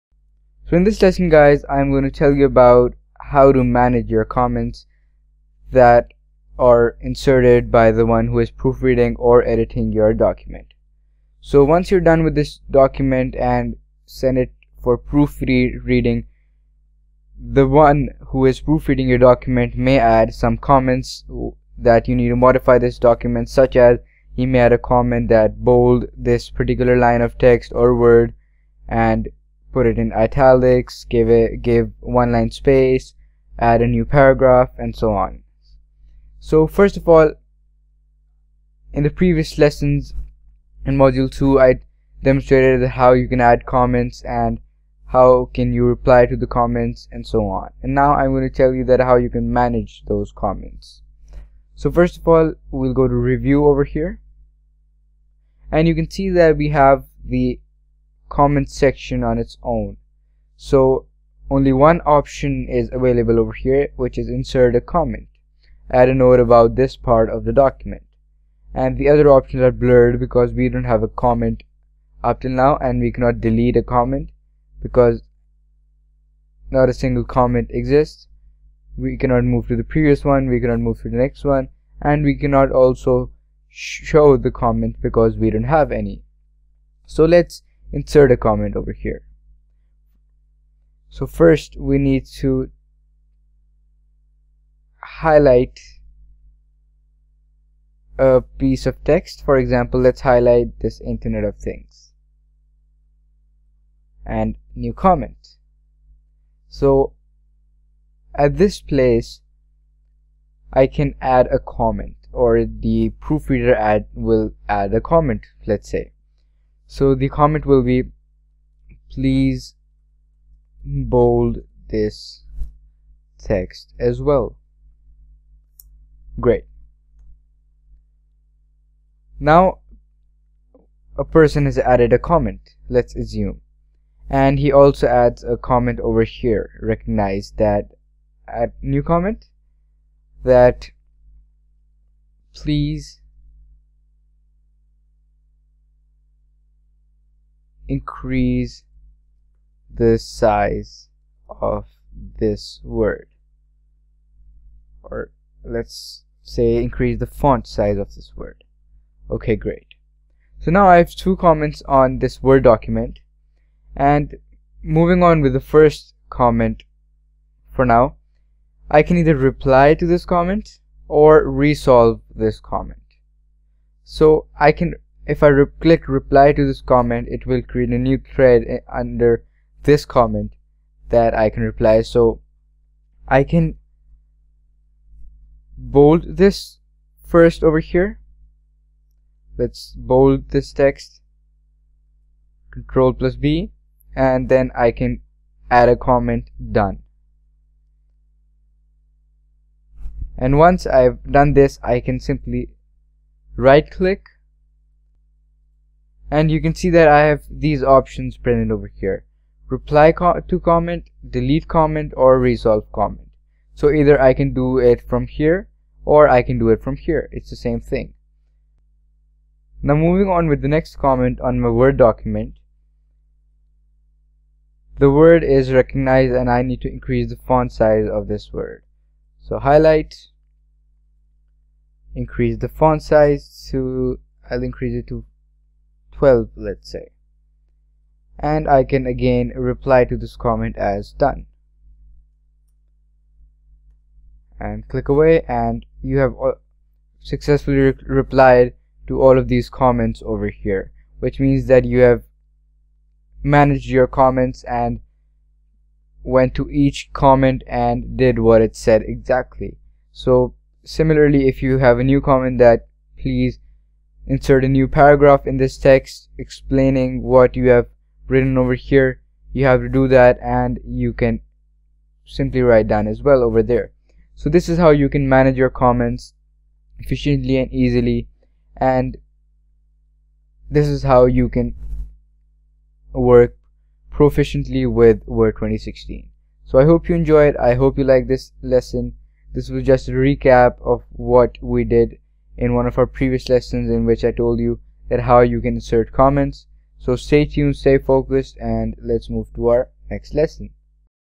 Managing Comments in Word 2016
Managing Comments in Word 2016. In this lesson you will learn how to manage comments inserted in word documents.